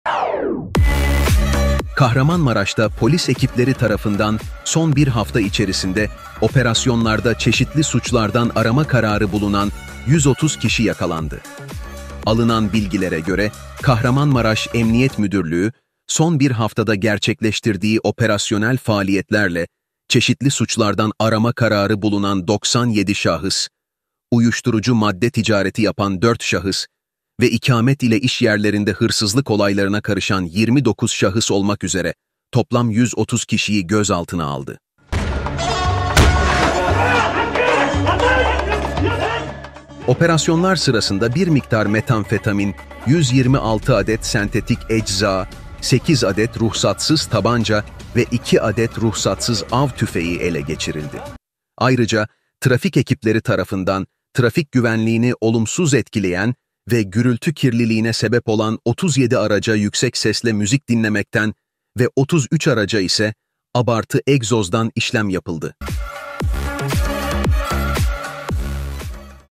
Kahramanmaraş’ta 130 Kişi Yakalandı!
Kahramanmaraş’ta polis ekipleri tarafından son bir hafta içerisinde operasyonlarda çeşitli suçlardan arama kararı bulunan 130 kişi yakalandı.
Alınan bilgilere göre Kahramanmaraş Emniyet Müdürlüğü, 05 - 12 Şubat 2024 tarihleri arasında gerçekleştirdiği operasyonel faaliyetlerle çeşitli suçlardan arama kararı bulunan 97 şahıs, uyuşturucu madde ticareti yapan 4 şahıs ve ikamet ile işyerlerinde hırsızlık olaylarına karışan 29 şahıs olmak üzere toplam 130 kişiyi gözaltına aldı. Gözaltına alınan ve adliyeye sevk edilen 130 kişiden 61’i tutuklanarak cezaevine konuldu.
Operasyonlar sırasında 44,38 gram metamfetamin, 126 adet sentetik ecza, 8 adet ruhsatsız tabanca ve 2 adet ruhsatsız av tüfeği ele geçirildi.
Ayrıca, trafik ekipleri tarafından, trafik güvenliğini olumsuz etkileyen ve gürültü kirliliğine sebep olan 37 araca yüksek sesle müzik dinlemekten ve 33 araca ise abartı egzozdan işlem yapıldı.